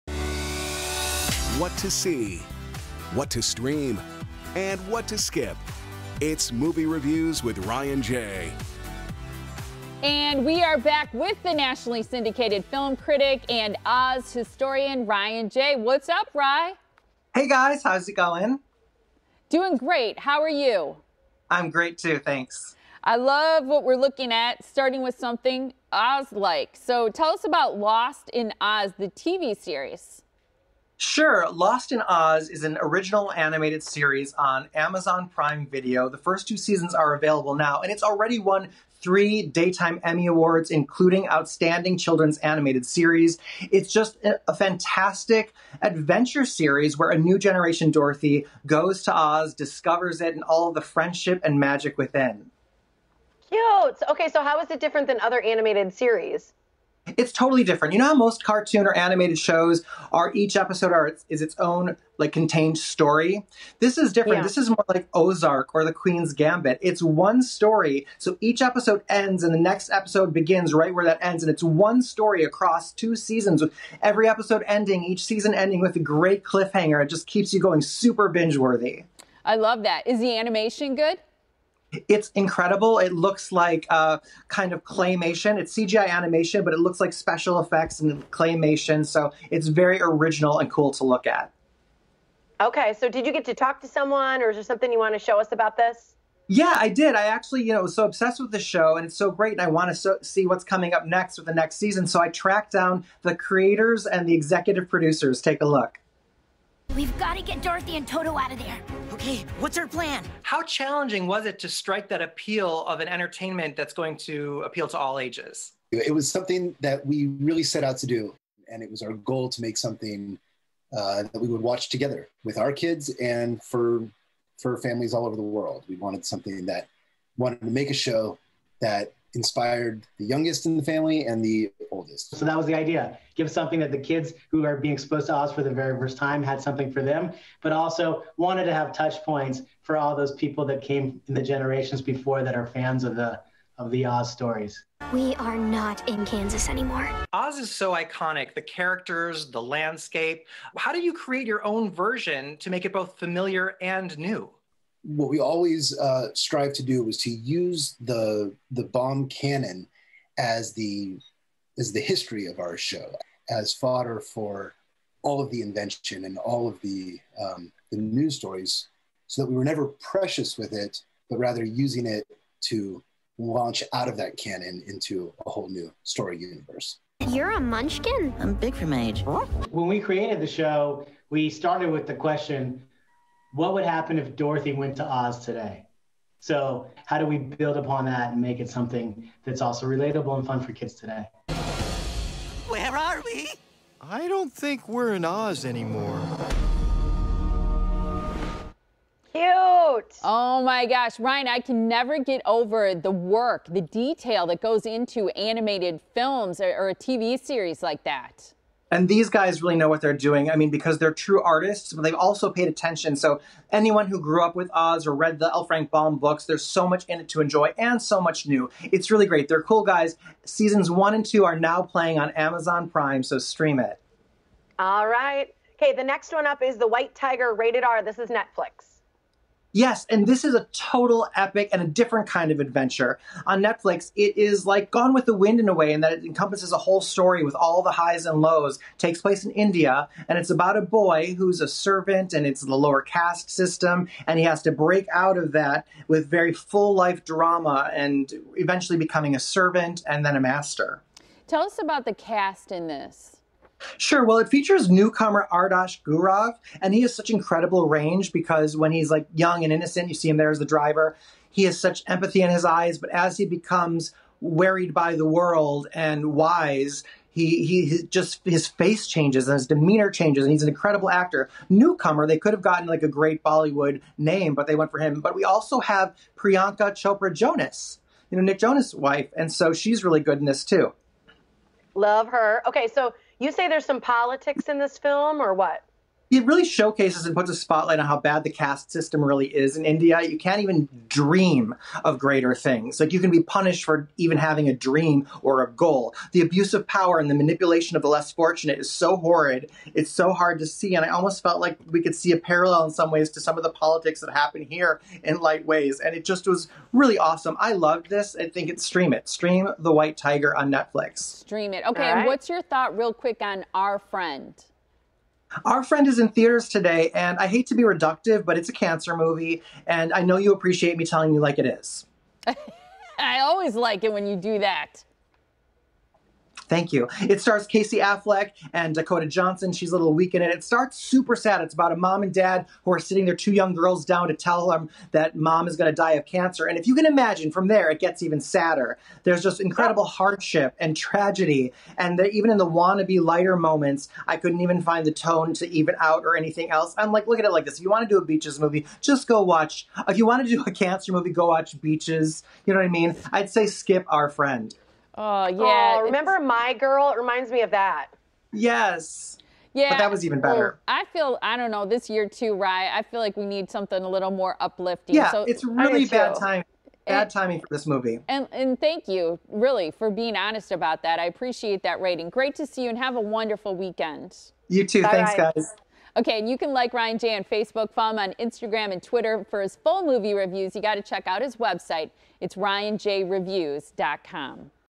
Celebrate the Weekend with a Great Movie!
What sounds better than ordering pizza and curling up to watch a good movie? Nothing! Syndicated Film Critic Ryan Jay agrees so he brought two new movies worthy of the ultimate movie night. Ryan's top picks for the week are:  Our Friend / Gravitas Ventures - rating TBD  The White Tiger / Netflix - Stream It  You can find more movies to pair with your favorite pizza at ryanjayreviews.com.